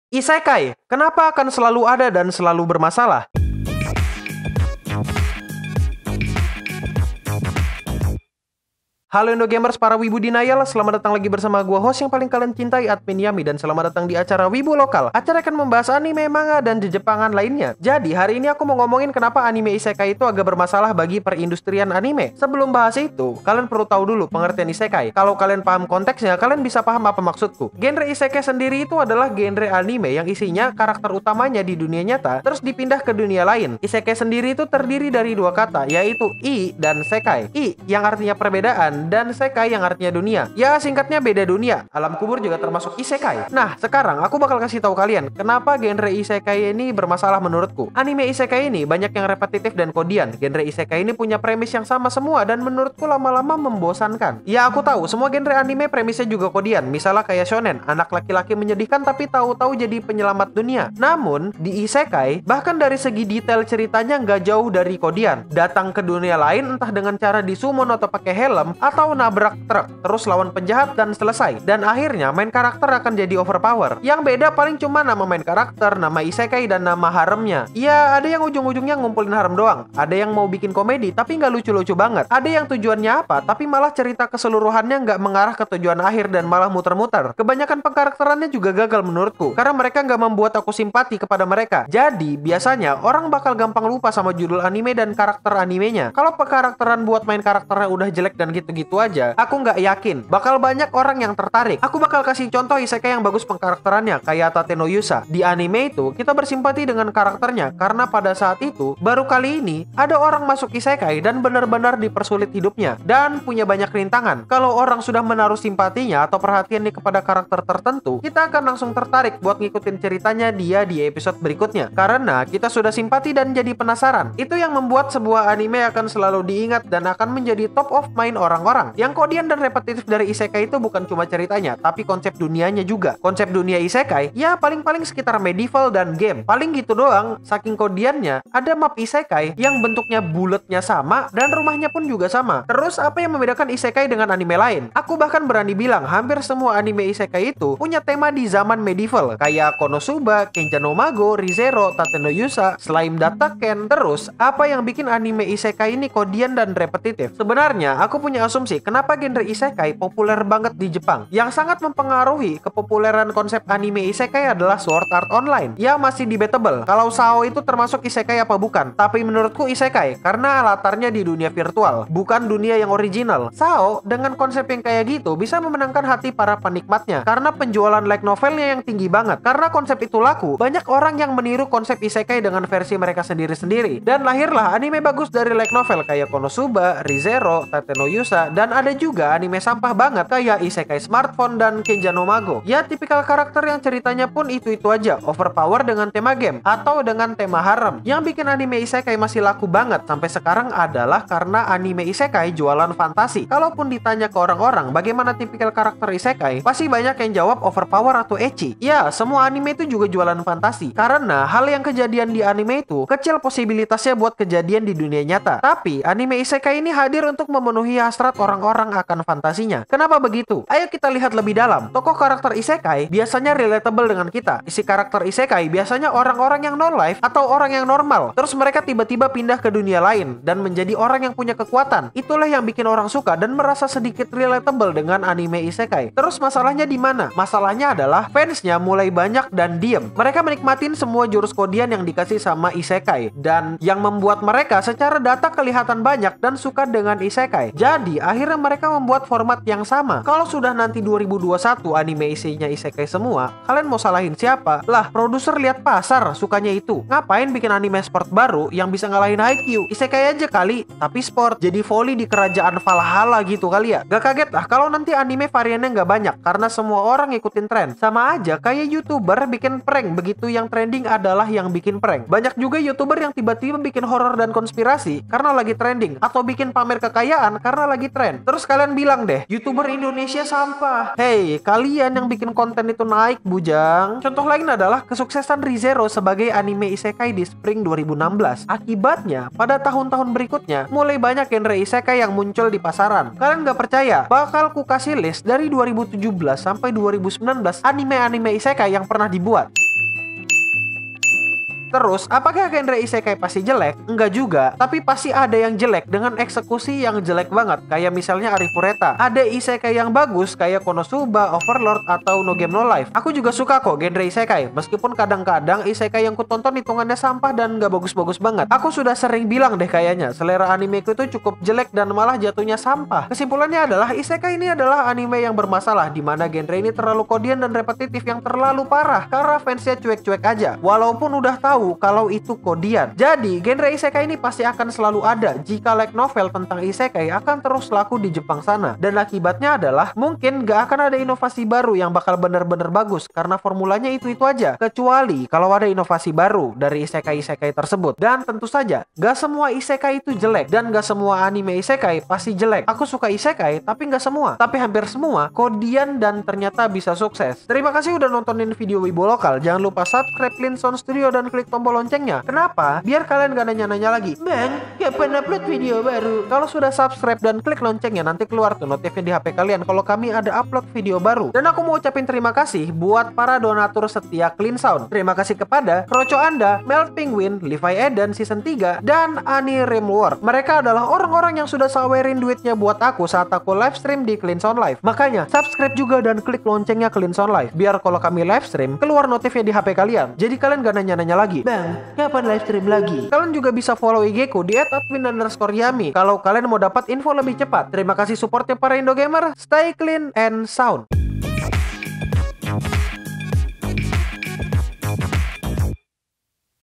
Semakin Kalian Benci Anime Isekali Semakin Isekai Beranak
Masalah pada Anime ISekai.

Untuk info mengenai endorsment, sponsorship, dan informasi lainya. Harap hubungi kami di:
Cindy Manager Cleansound Studio: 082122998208